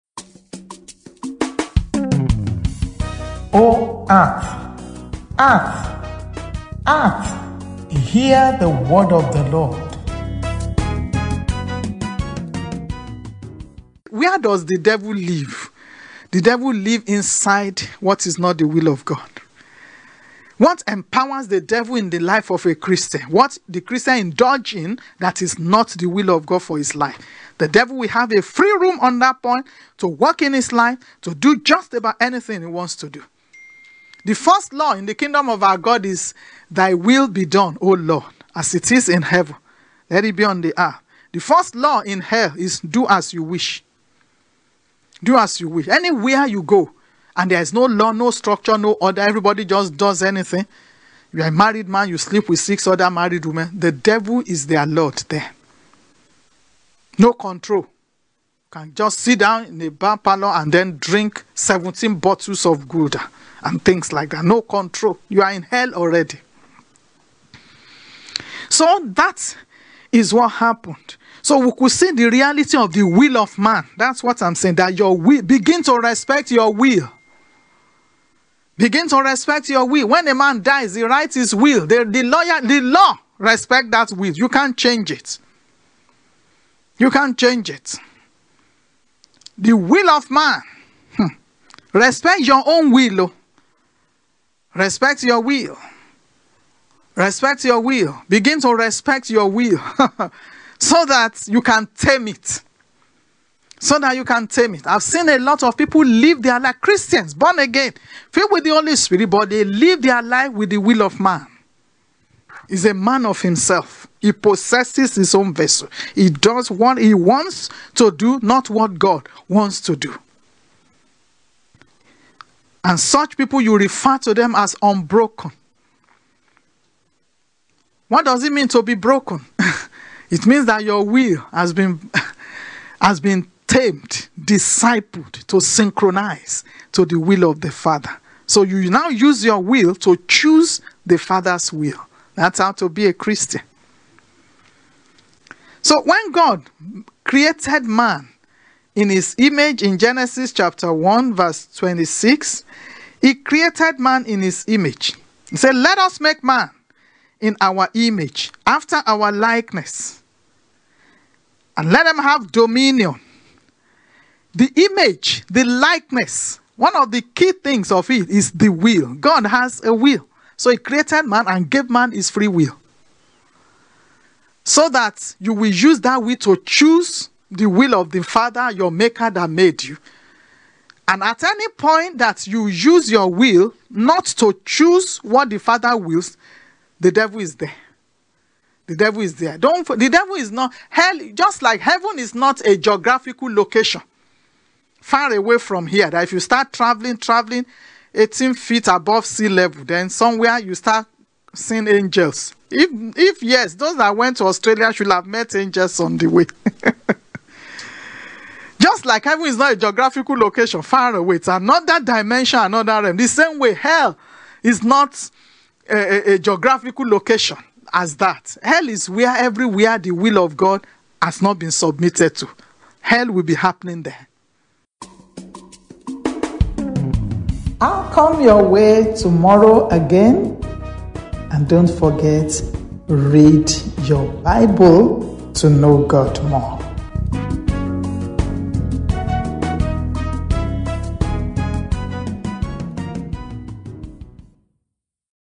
Episode 015- "O earth, earth, earth, hear the word of the LORD" series- Rev Oyenike Areogun
O earth, earth, earth, hear the word of the LORD series
Episode 015: You are as safe as you are in the will of God!- Rev Oyenike Areogun
Awakening the world to hear God's word in order to experience the supernatural.